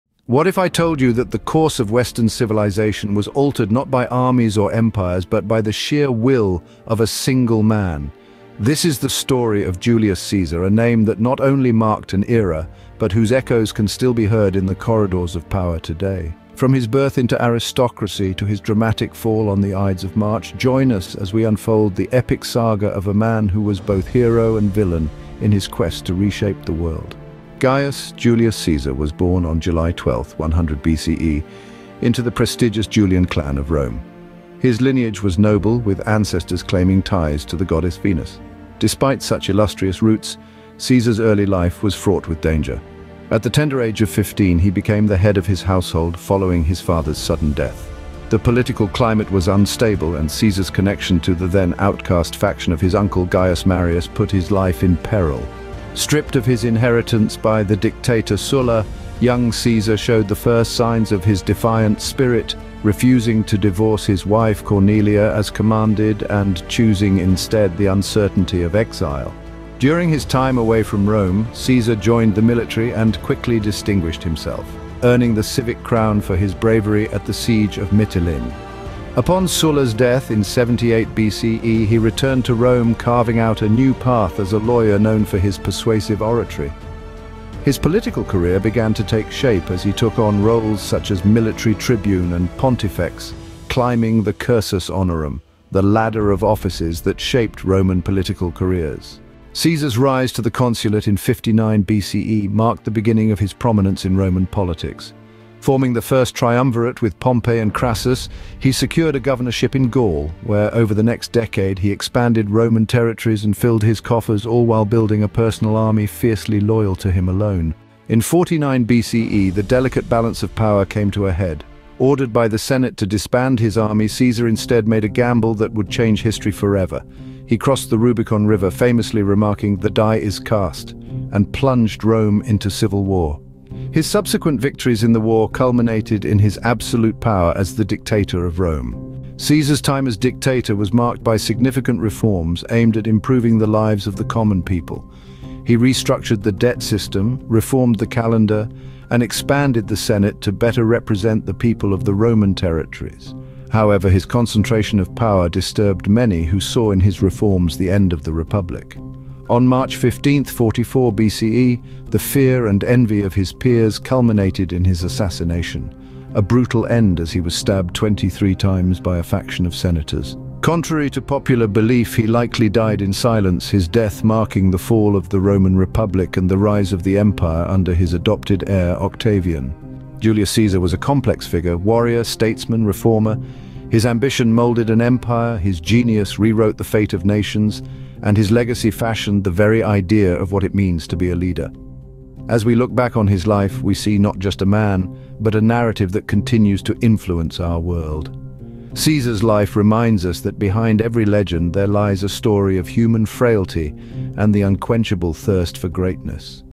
Julius Caesar: The Man Who Changed Rome Forever
Dive into the dramatic and pivotal life of Julius Caesar in this short documentary. From his early struggles and exile to his unprecedented rise to power and sudden assassination, discover how one man's ambition reshaped the Roman Republic and paved the way for the Roman Empire. Watch as we explore his military conquests, political reforms, and enduring legacy that still influences the modern world. Whether you're a history buff, a student, or just curious about one of history's most iconic leaders, this video is for you!

0:00 Intro
00:00:27 The Early Life of Julius Caesar
00:01:09 Military Career and Political Ascent
00:01:39 Rise to Power and the First Triumvirate
00:01:57 Crossing the Rubicon and Dictatorship
00:02:19 Caesar's Reforms and Assassination